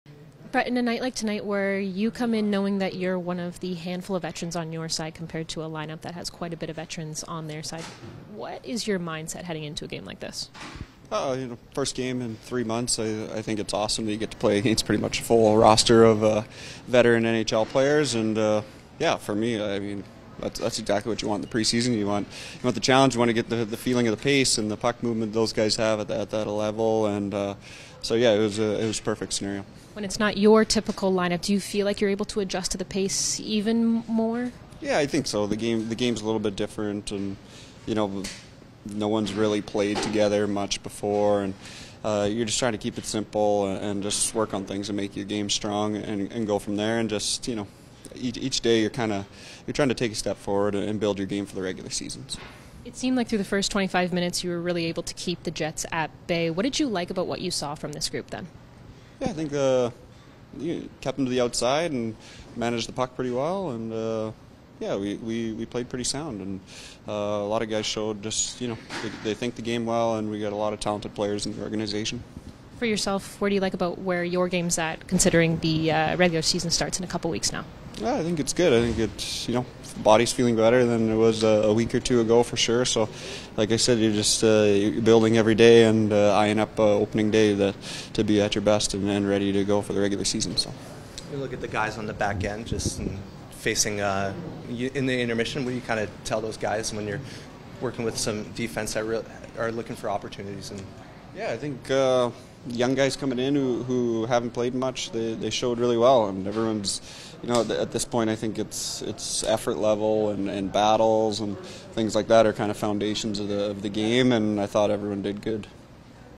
POST-RAW | Brett Kulak 09.25.24
Brett speaks after the Oilers preseason defeat in Winnipeg on Wednesday.